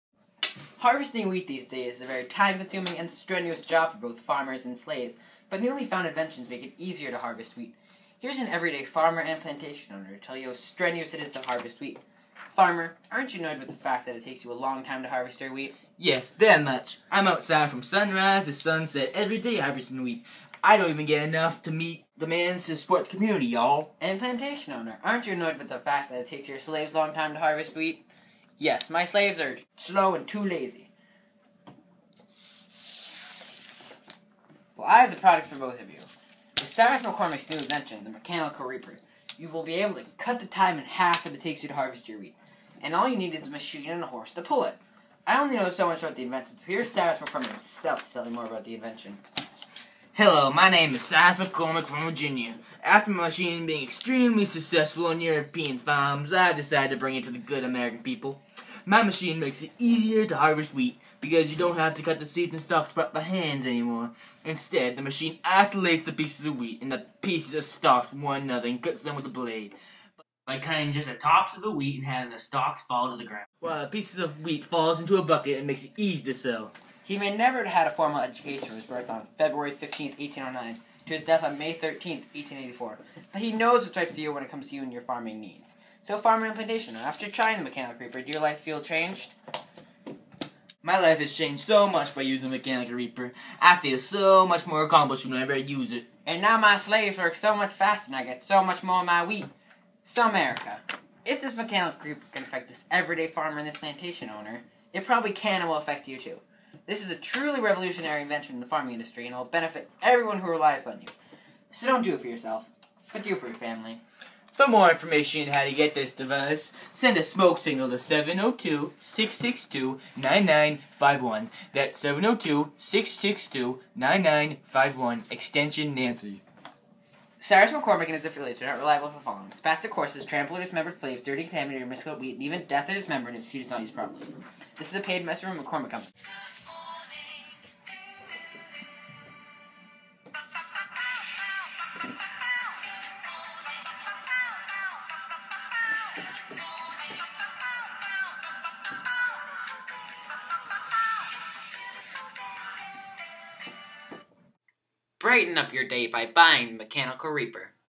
Cyrus McCormick F Block Rob and Mason
Mr. Oostings F Block: Robert Lesch and Mason Iriana's presentation on The Mechanical reaper.